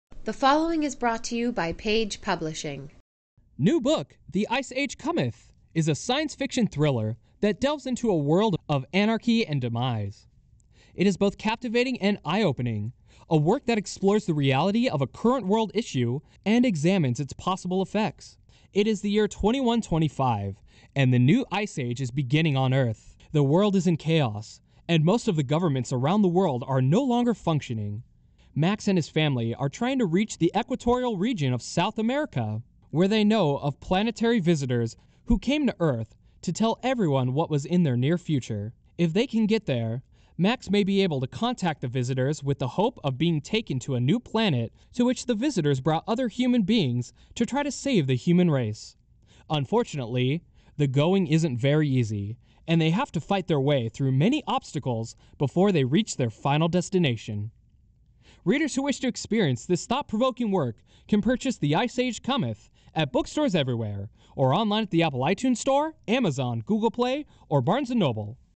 The Ice Age Cometh by Robert Stach
The Ice Age Cometh

It is now the year 2125 and the ice age about which the people on Earth were told is actually starting. The world is in chaos and most of the governments around the world are no longer functioning. The Washburn-Melbanks family, which includes Max and his wife Alice, their two twin daughters, and both sets of grandparents are trying to reach the equatorial region of South America. Max knows that the visitors who came to Earth to tell everyone what was in their near future made a short stop at the equatorial region in the year 2130. If they can get from Minnesota to Colombia and the equatorial region, they may be able to contact the visitors with the hope of being taken to a new planet to which the visitors brought other human beings to try to save the human race. Unfortunately, the going isn’t very easy and they have to fight their way through many obstacles before they reach their final destination. Even though they do eventually reach their goal, will they be able to contact the visitors and be taken by them to the new world where other human beings are now living.

--Robert Stach

Page Publishing Inc.